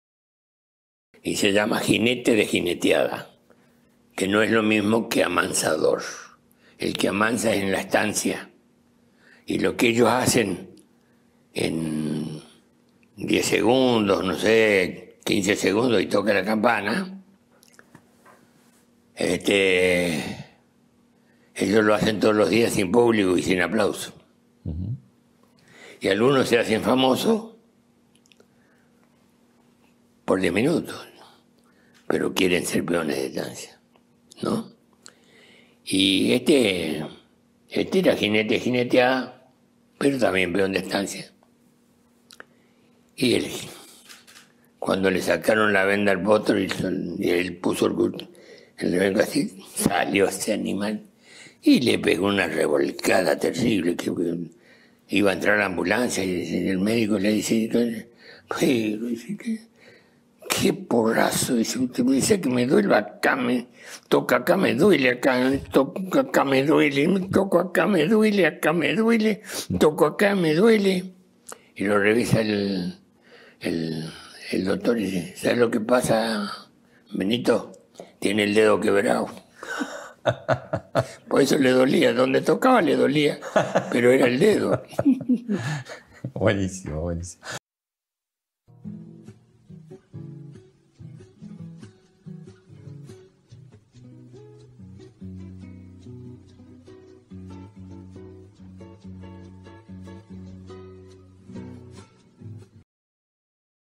JINETE DE JINETEADA | LUIS LANDRISCINA
LUIS LANDRISCINA en una reciente entrevista en LN nos cuenta "Jinete de Jineteada" la diferencia con la Doma y el Domador.